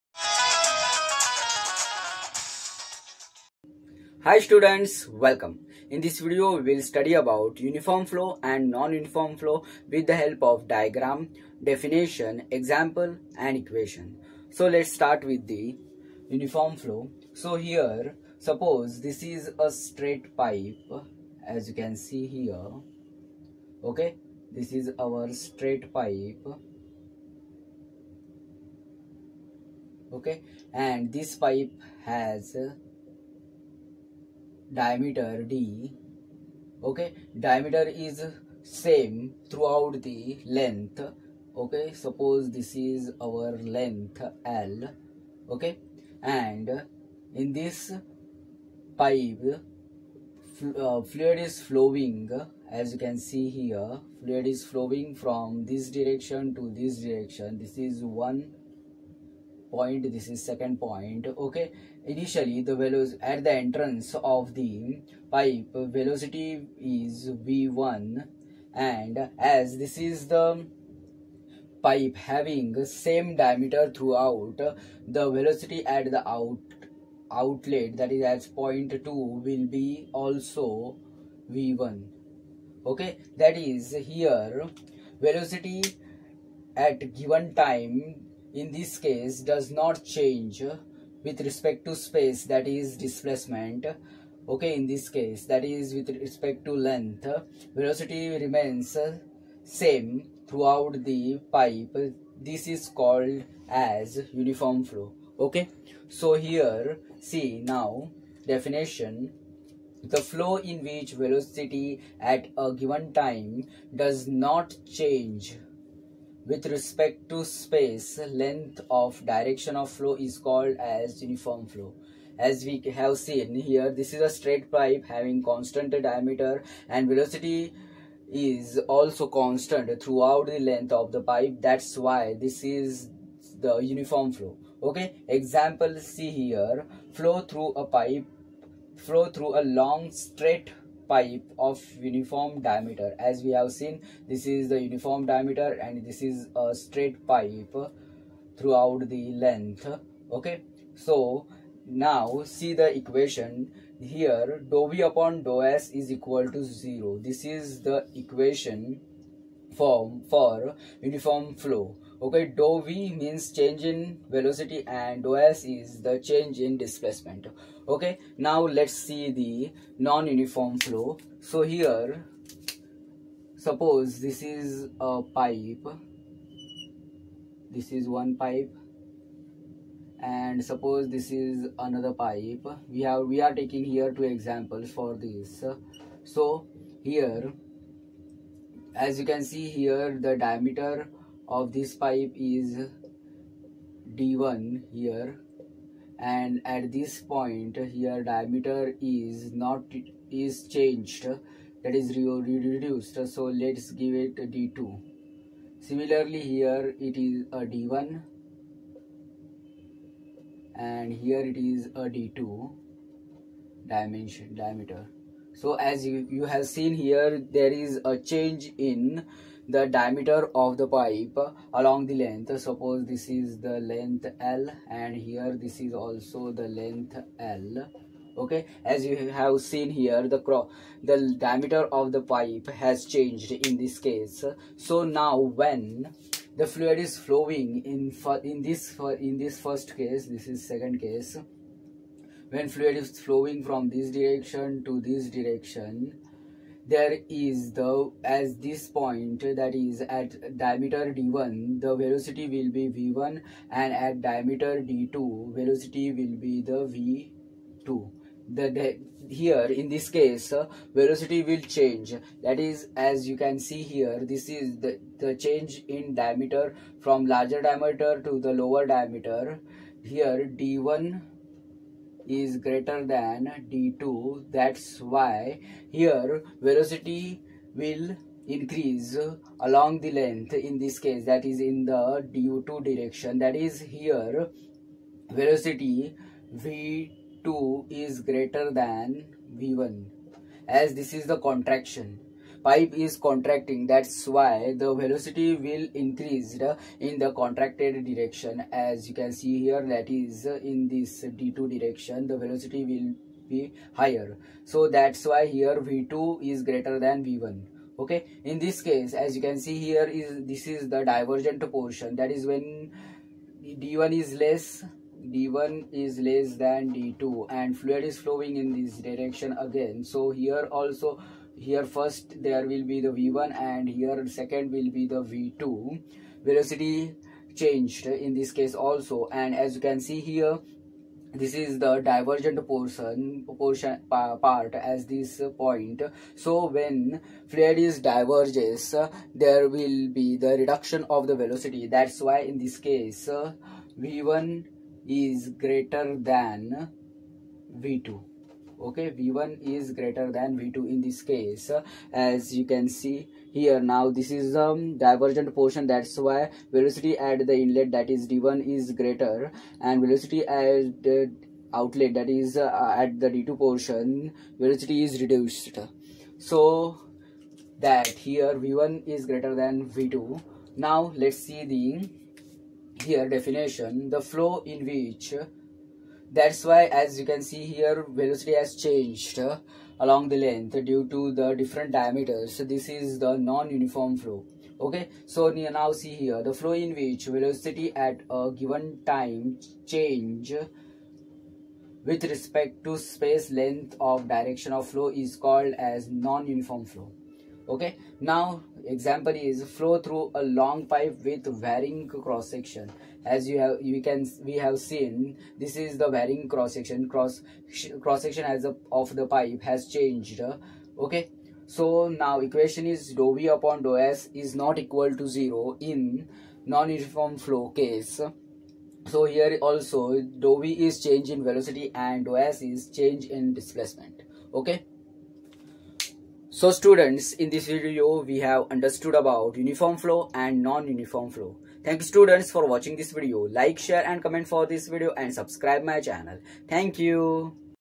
Uniform Flow and Non Uniform Flow | Fluid Mechanics
I am Pratik Pagade.

Video Description :-
The video will help you to understand the difference between uniform flow and non uniform flow.

1) What is uniform Flow and non uniform Flow ?
2) What is uniform Flow fluid mechanics ?
3) What is non uniform Flow fluid mechanics ?
4) What is the difference between uniform Flow and non uniform Flow ?

Pratik Pagade
PSP